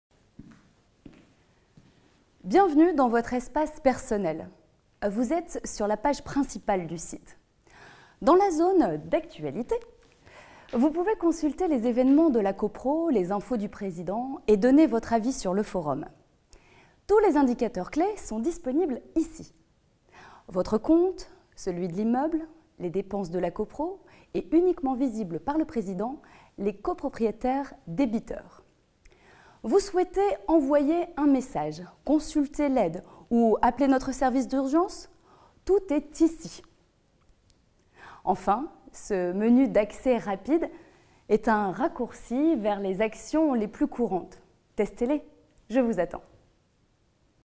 SYNDIC ONE
Bienvenue dans votre espace personnel.
Vous êtes sur la page principale du site.
Dans la zone actualités, vous  pouvez consulter les évènements de la copro , les infos du président et donner votre avis sur le forum !

Tous les indicateurs clés sont disponibles ici : votre compte, celui de l’immeuble, les dépenses de la copro et uniquement visible par  le président, les copropriétaires débiteurs !

Vous souhaitez  envoyer un message, consulter l’aide ou  appeler notre service d’urgence ? Tout est ici

Enfin, ce menu d’accès rapide est un raccourci vers les actions les plus courantes Testez-les ! Je vous attends    …

"Aidez Sofian dans son combat contre l'ataxie telangiectasie"